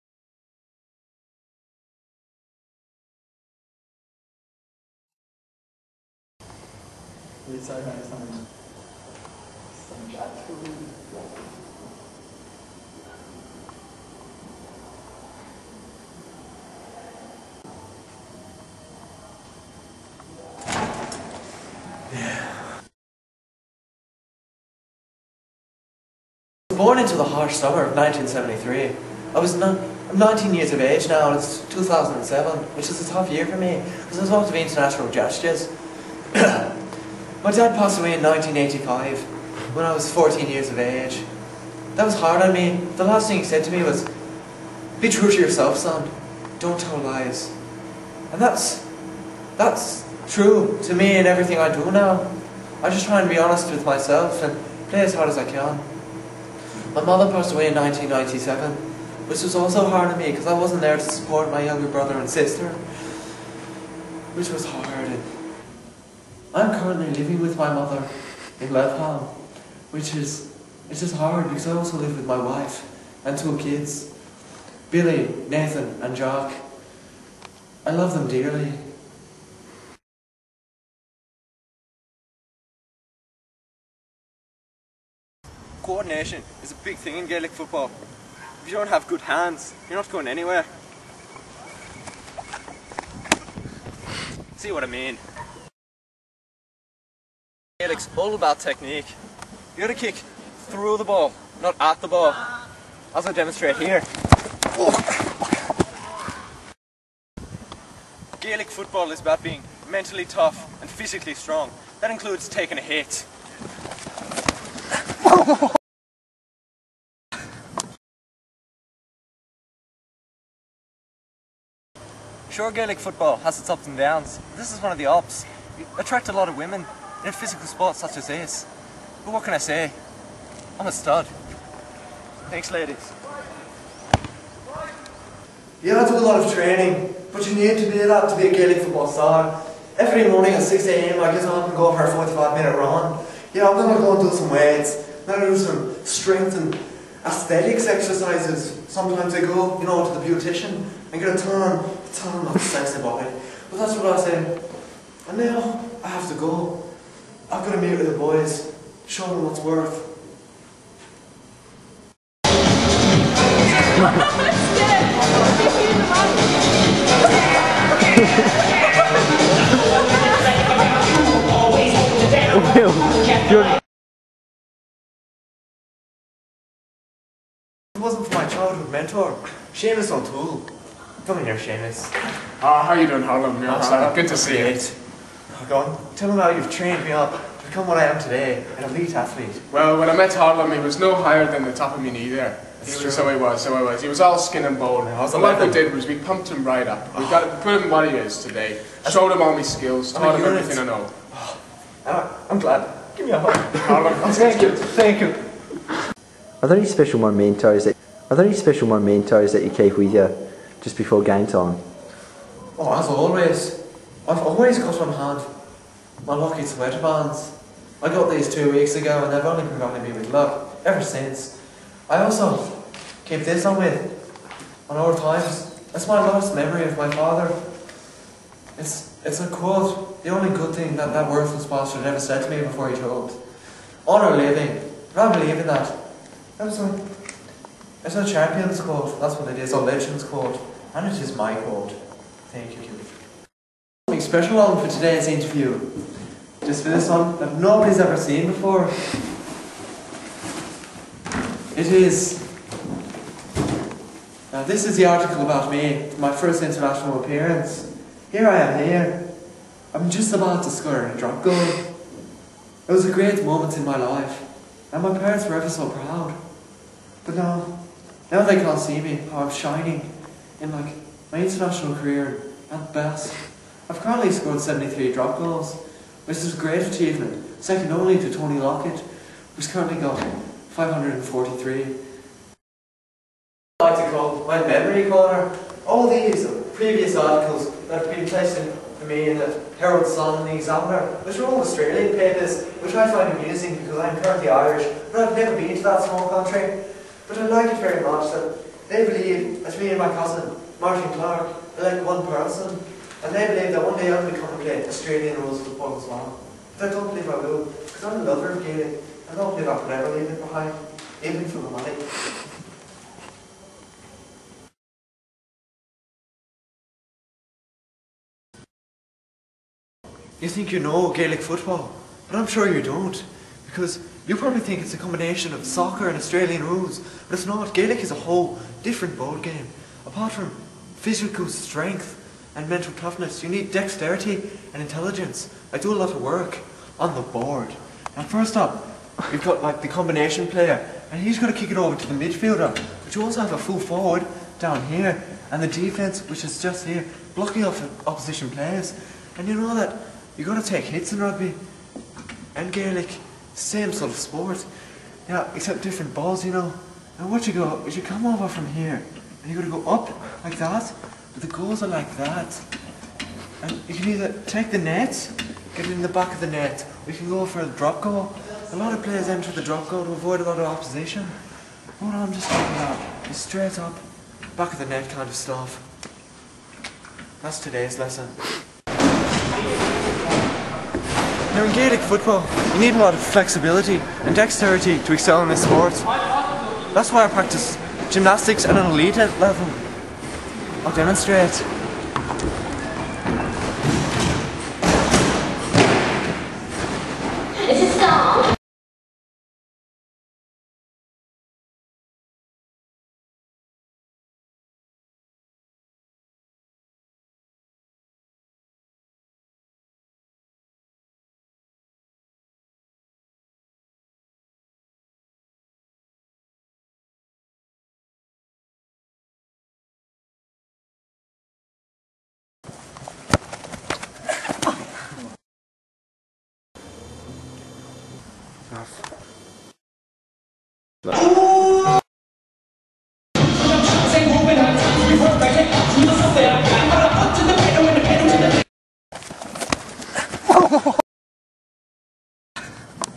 Honour Living.
Documentary on Harlem Marks - A high profile figure in the world of gaelic football.